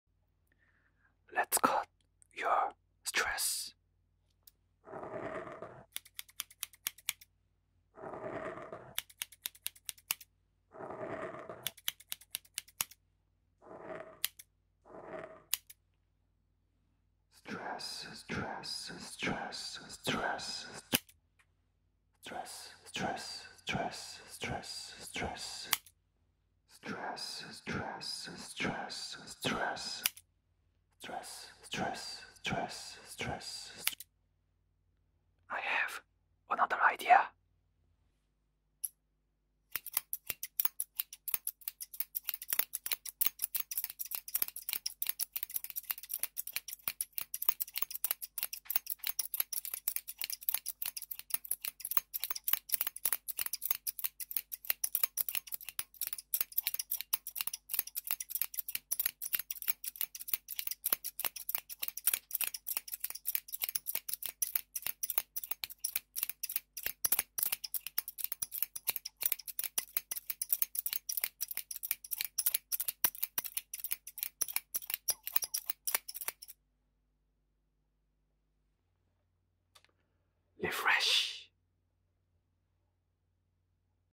ASMR Haircut for Stress Relief (Layered Sounds) ✂
Hello, I’m SCAHN.
I made haircut sounds video.
I used 2 types of scissors.
It’s more than 1 minute(and less than 90 seconds).
I hope you feel cutting your stress
and refresh your mood by this sounds^^!!